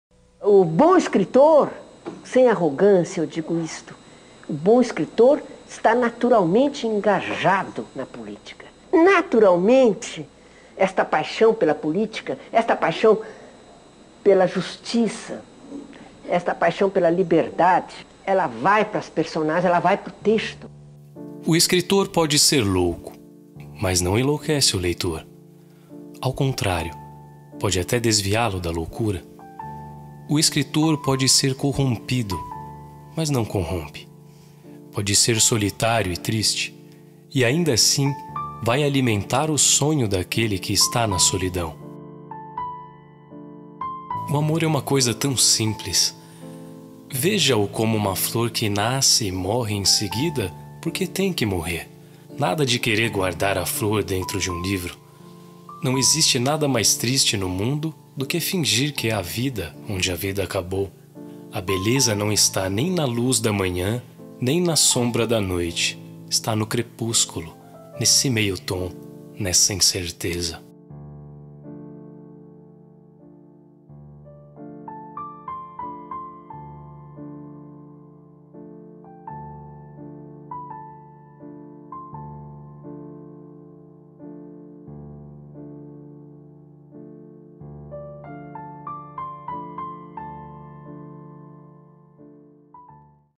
ELA ESTÁ VIVA NAS PALAVRAS - LYGIA FAGUNDES TELLES
Lygia Fagundes Telles, escritora brasileira, nasceu em 19 de abril de 1923, em São Paulo.
Publicou seu primeiro livro de contos "Porões e sobrados" em 1938.
Formada em Direito, trabalhou como procuradora do Instituto de Previdência do Estado de São Paulo, foi presidente da Cinemateca Brasileira e fez parte da comissão que, em 1977, entregou ao ministro da Justiça o Manifesto dos intelectuais, abaixo-assinado contra a censura.
Lygia morreu em casa aos 98 anos de causas naturais.

Os textos presentes no vídeo, são trechos retirados de seus livros.